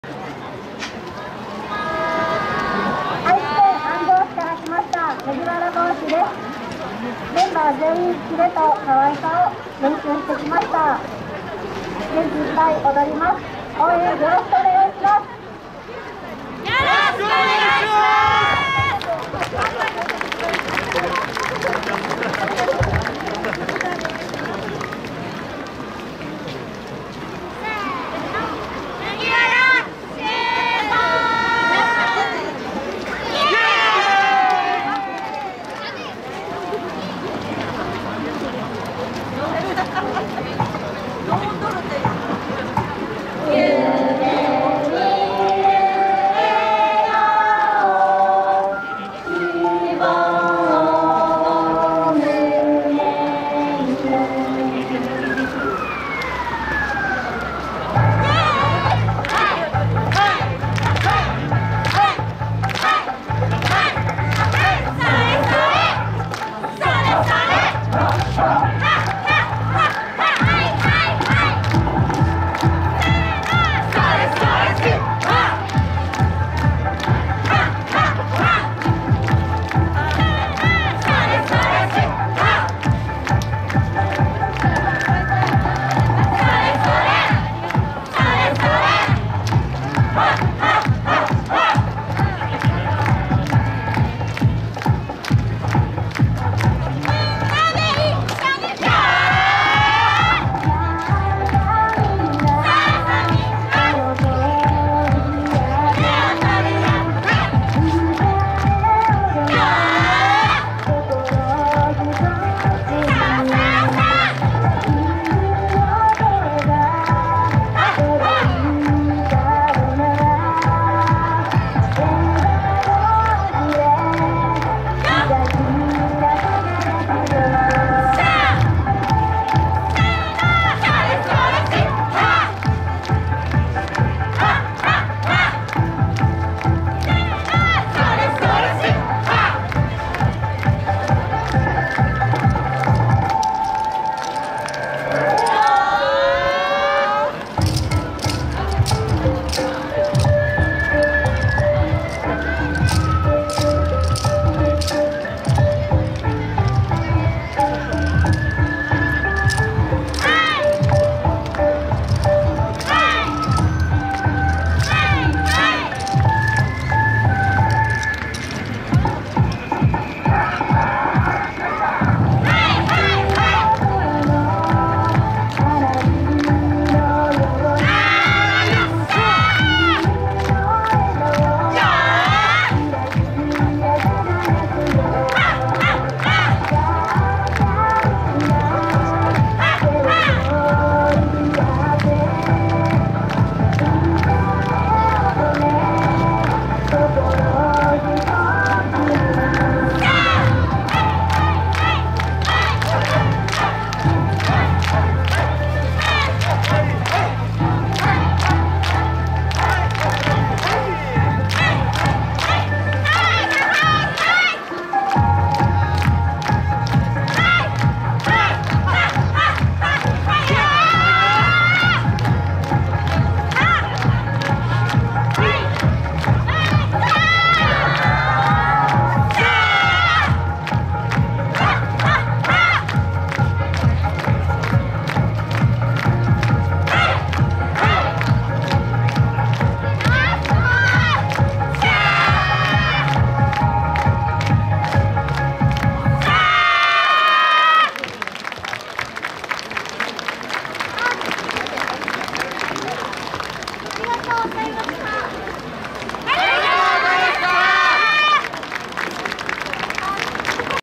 夢希笑～Boshi～ 福鬼まつり 2023 2回目 2月5日
福鬼まつり にて 　夢希笑～Boshi～　2回目　の演舞を撮影させていただきました。